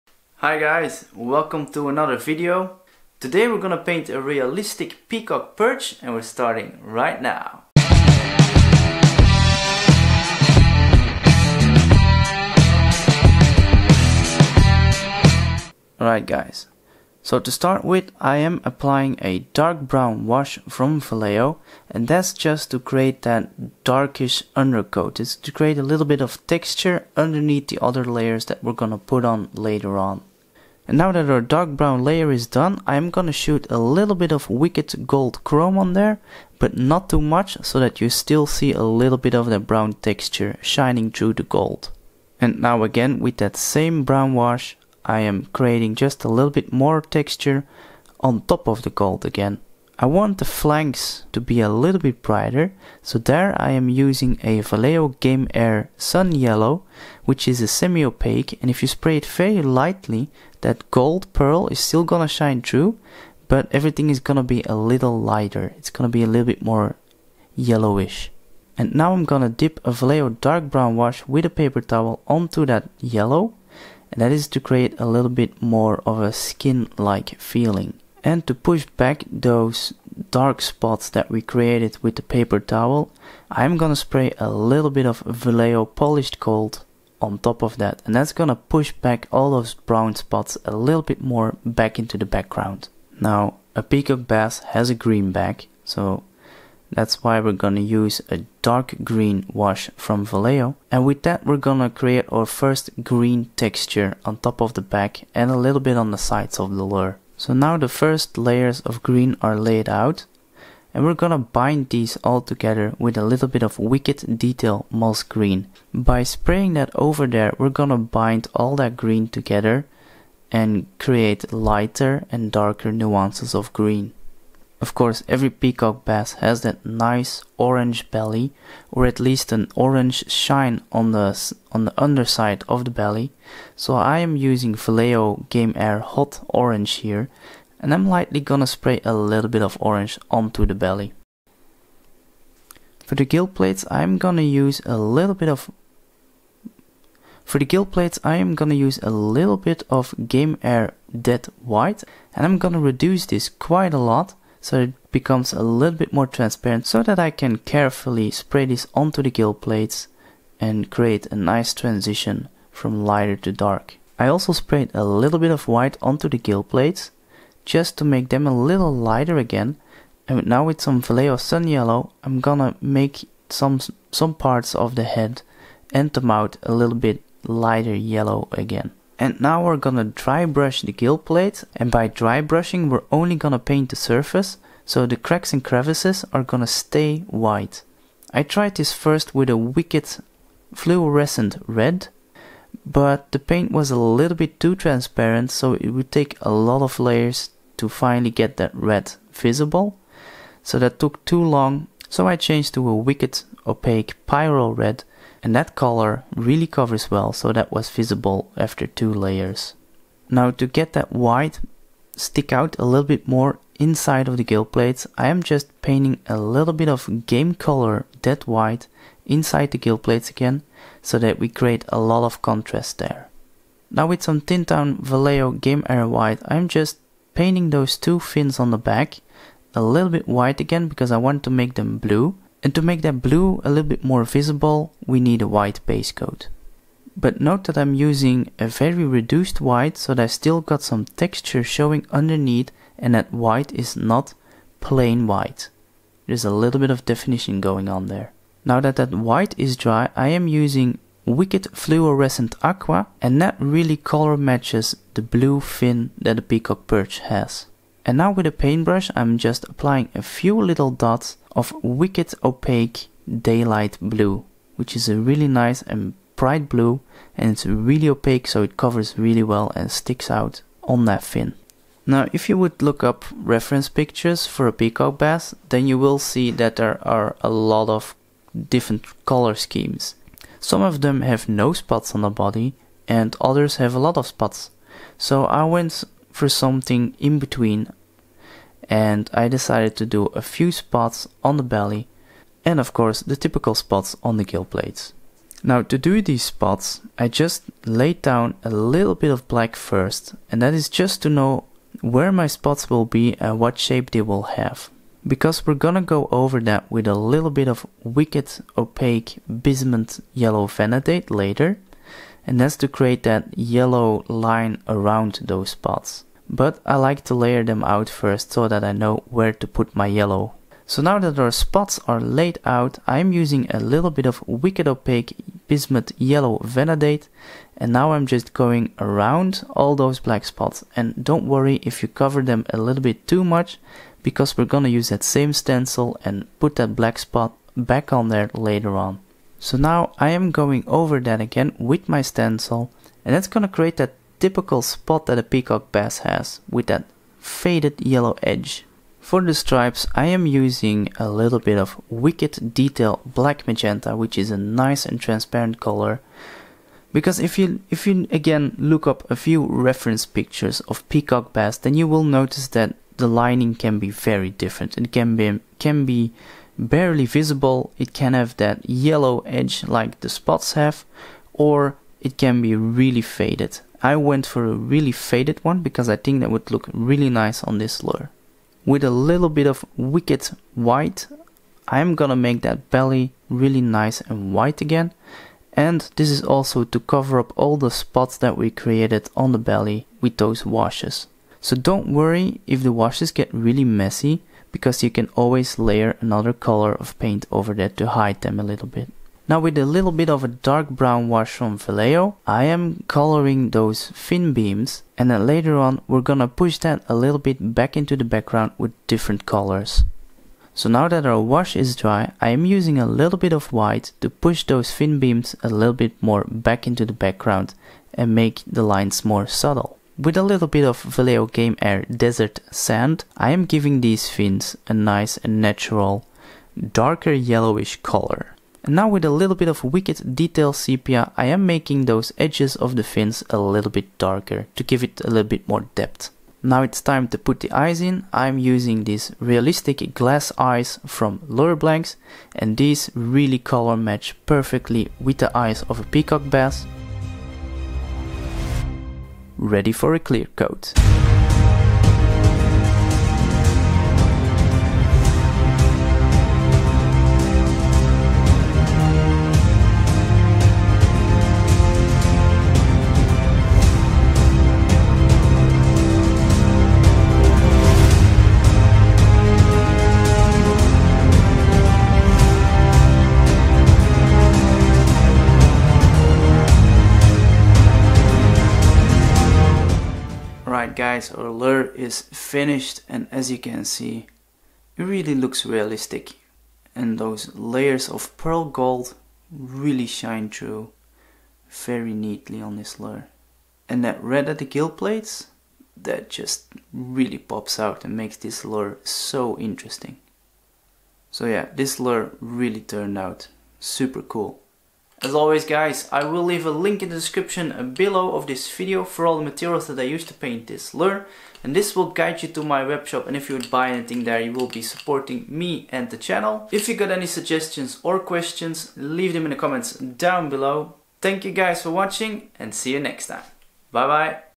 The ULTIMATE Peacock Bass Lure! How to Paint a Lifelike Bait they CAN'T Resist
Lure painting a realistic Peacock Bass pattern on a glide bait blank.

Follow us also on Instagram and Facebook for more content, products and fellow lure painters/makers their creations!